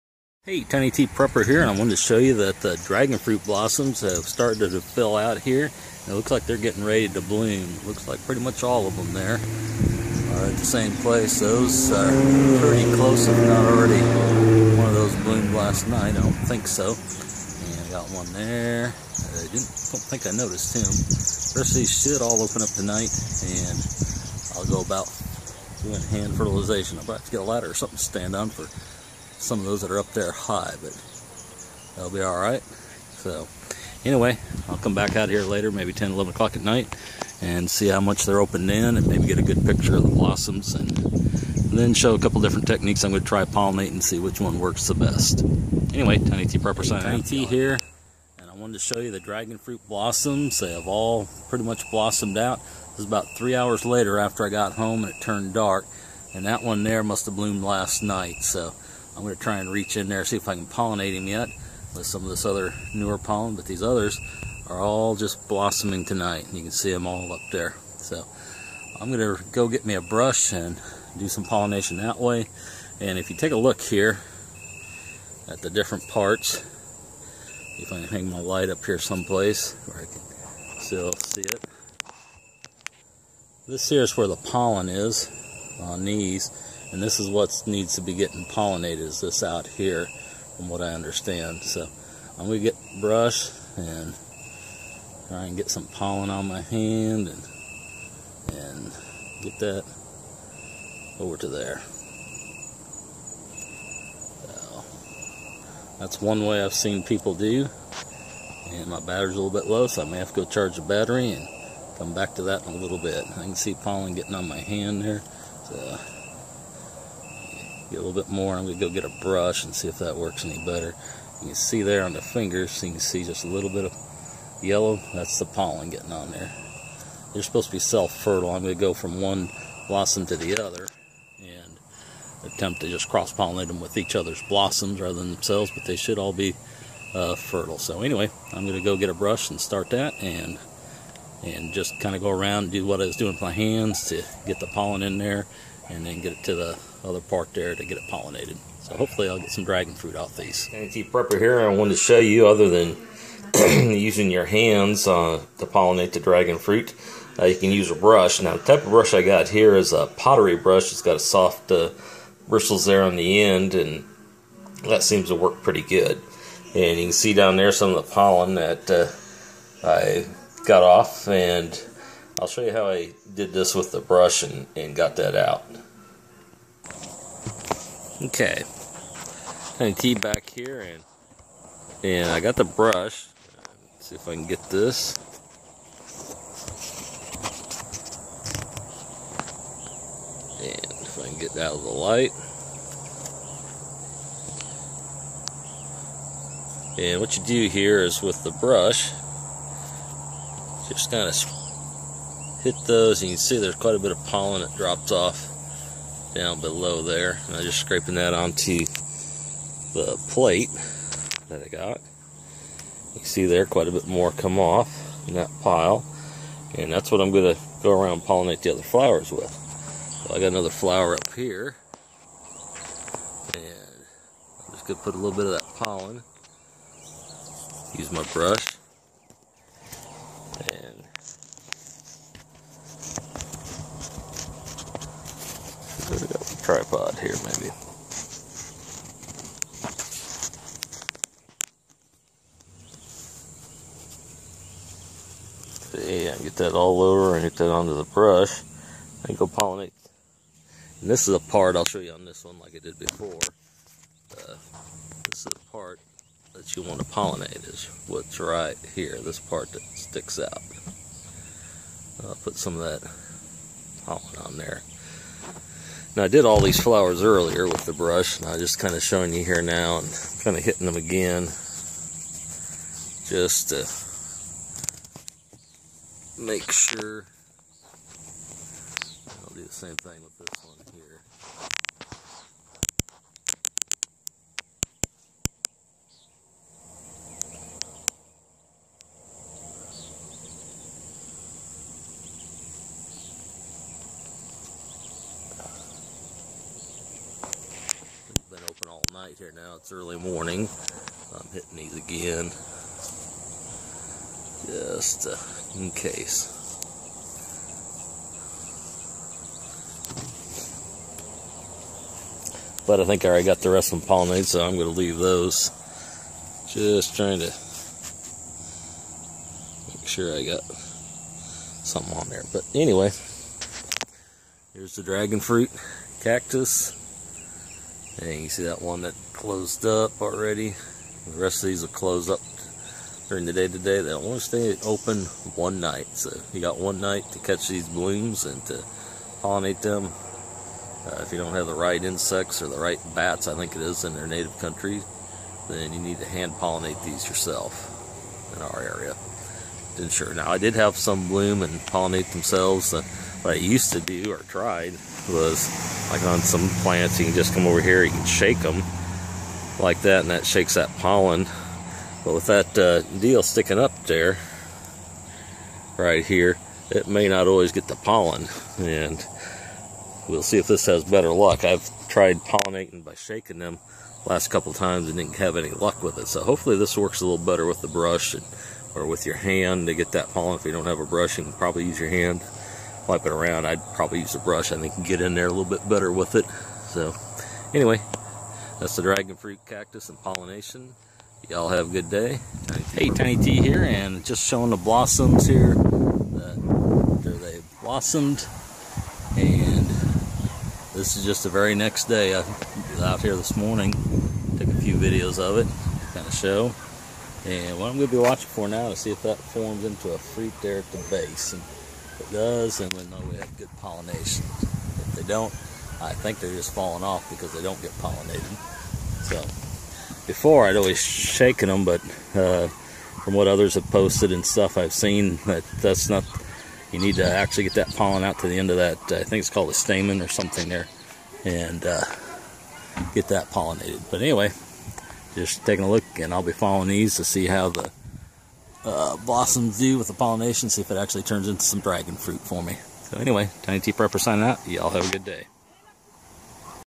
Hand Pollination of Dragon Fruit Blossoms
Had several dragon fruit blossoms.  Some of the previous blossoms never produced any fruit so I decided to hand pollinate these in an attempt to increase the success of fruiting.